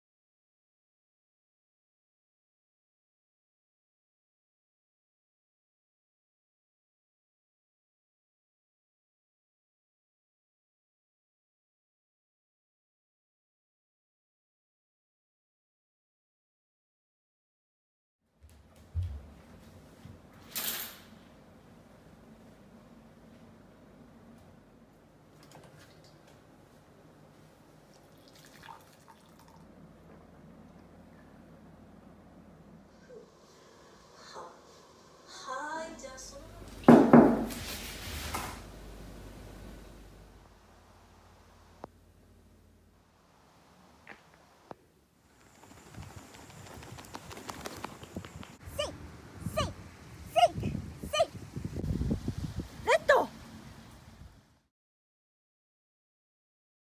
岩手の戦隊ヒロイン！？女性美戦隊エスメラルダーseason2#岩手県
エスメラルダベリーダンススクールの秘密組織。
女性美戦隊エスメラルダー！！
season2
…REDの復活　復帰のR！